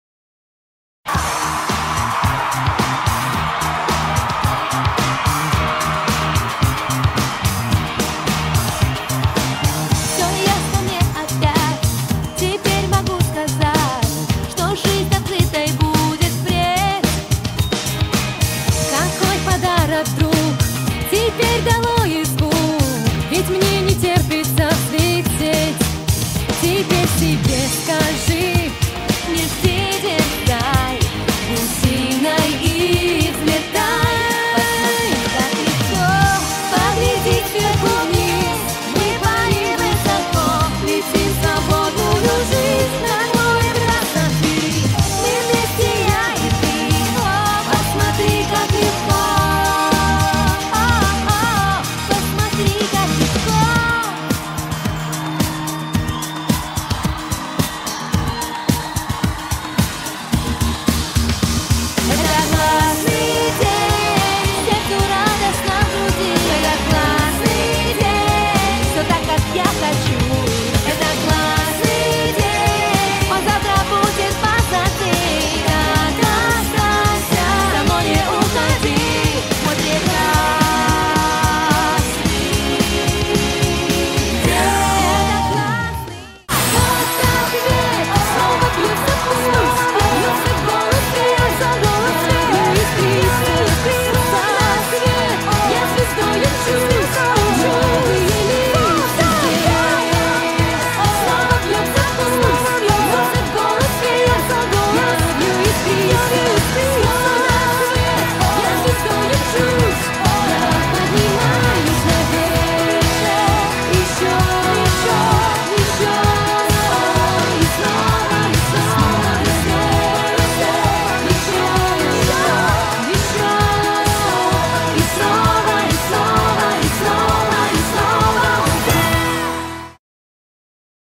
Финальная песня
Все кончилась хорошо подружки снова стали собой и теперь на стал концерт пара петь.